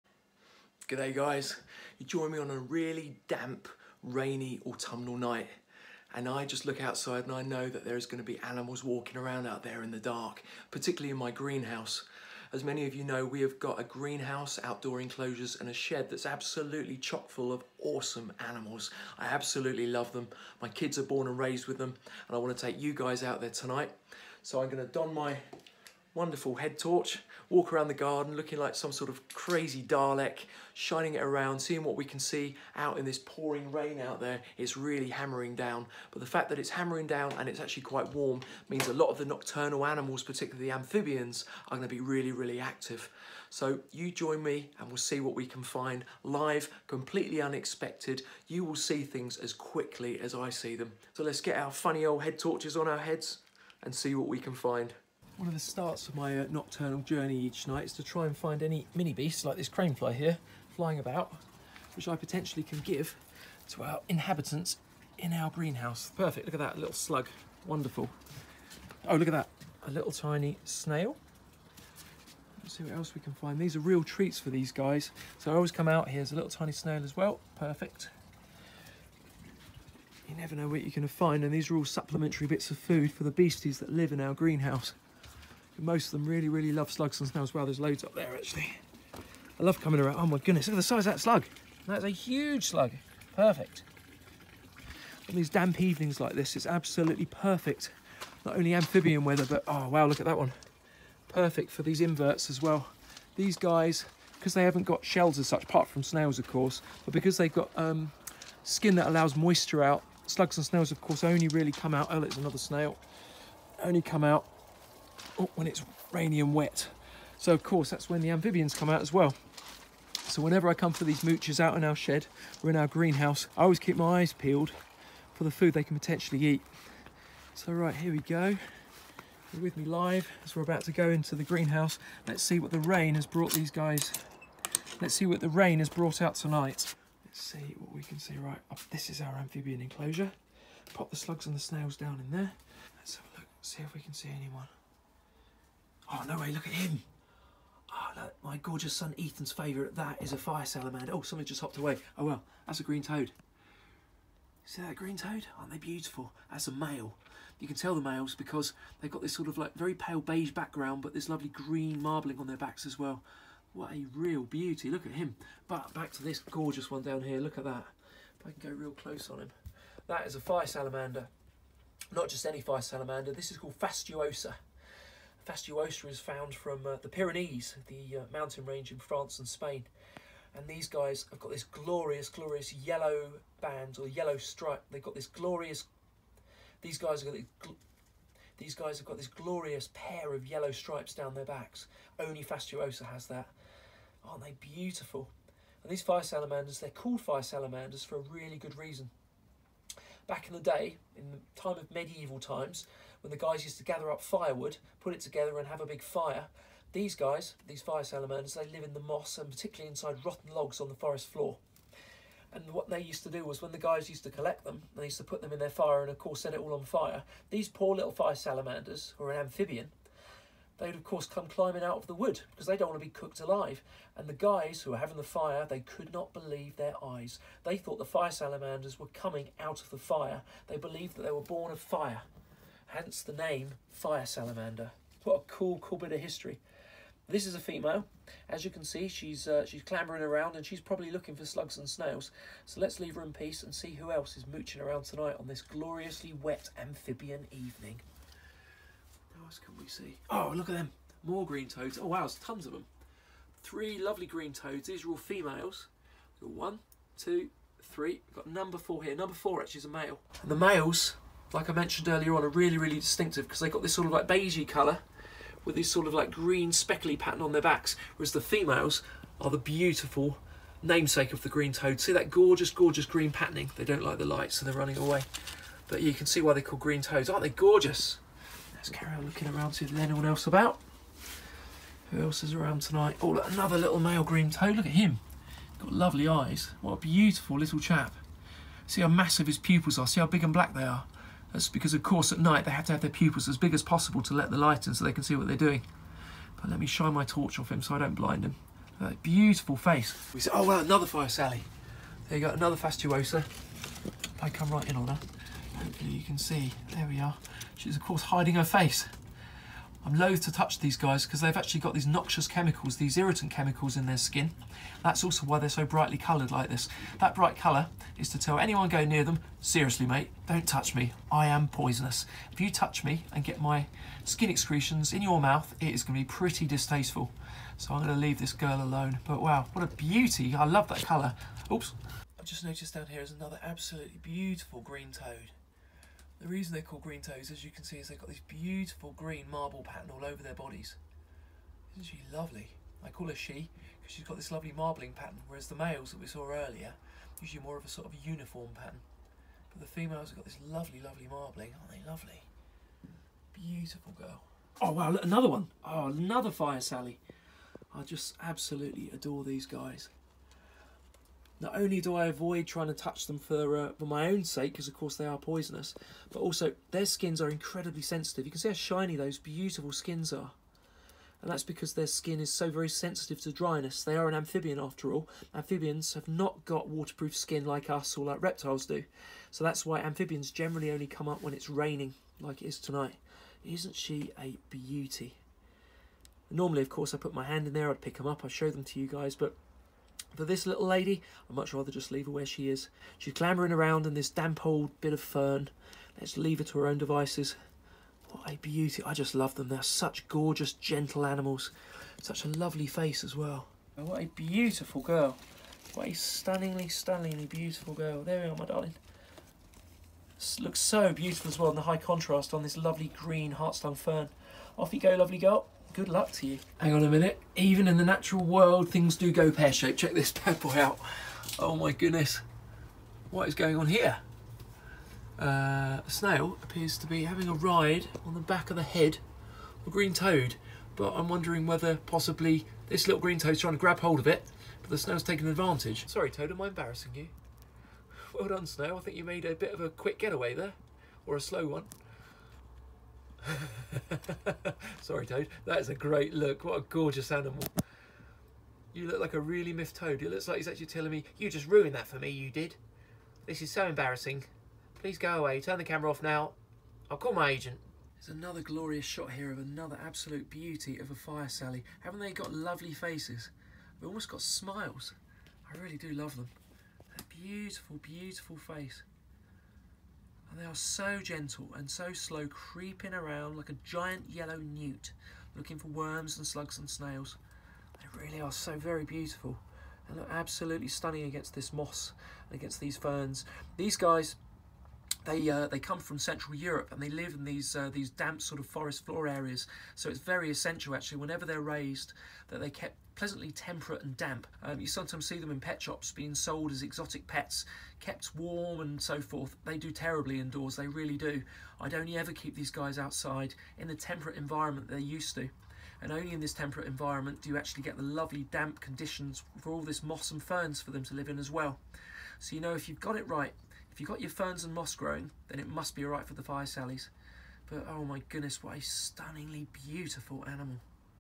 Amphibian Safari Part 1
Join me live on a damp night as I take you into my greenhouse to see what exotic amphibians are out and about in the gloriously perfect amphibian weather, enjoy